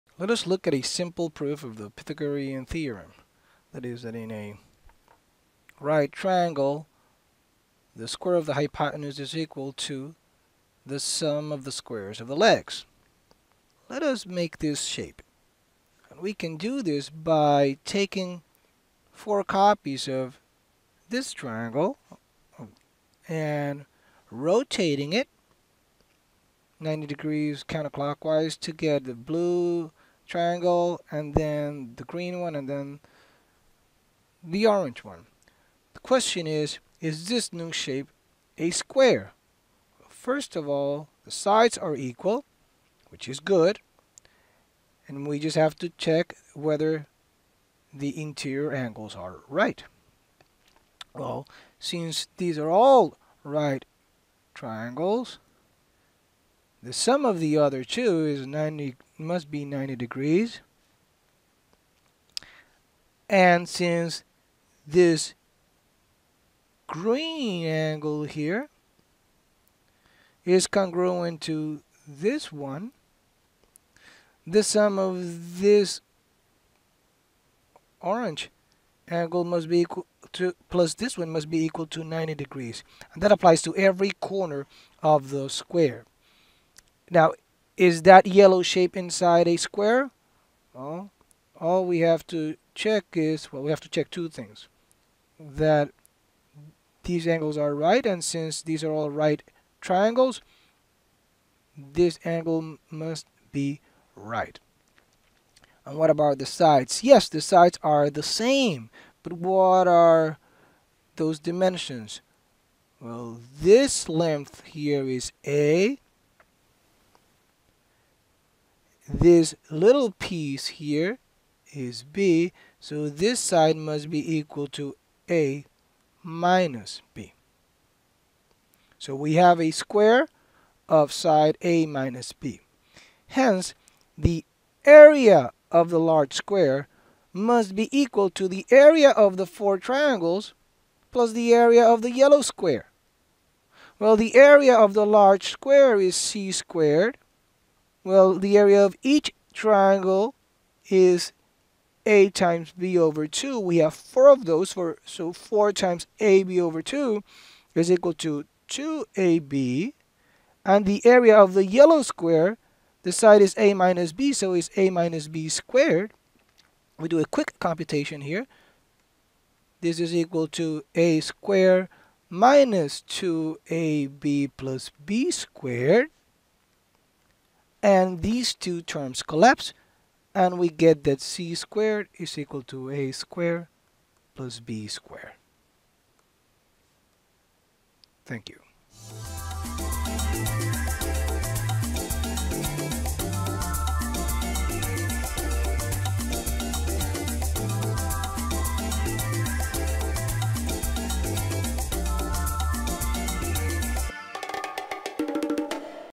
The Pythagorean Thoerem
A demonstration of one of the simplest proofs of this famous result, which resembles Bhaskara’s proof.

Go here for guiding questions, notes for teachers, and example problems that you can try